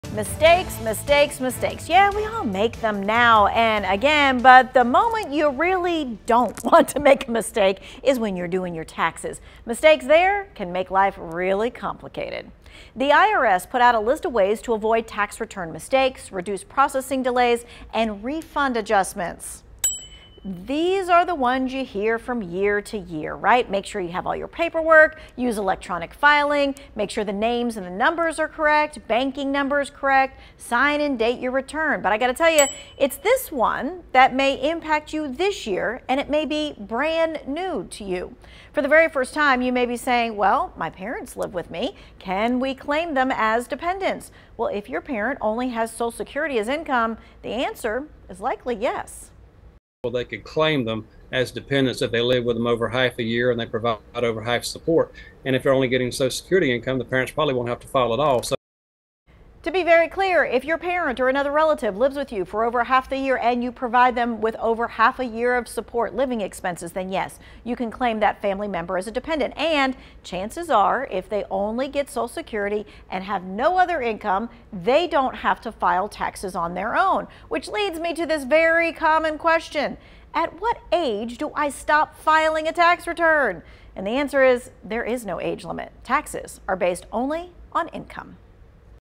Who can you claim as a dependent on your taxes?
We all make mistakes now and again, but the moment you want to avoid making mistakes is when you do your taxes. The IRS lists ways to prevent tax return mistakes and reduce processing delays and refund adjustments.

There are several common ways to avoid mistakes that you hear every year: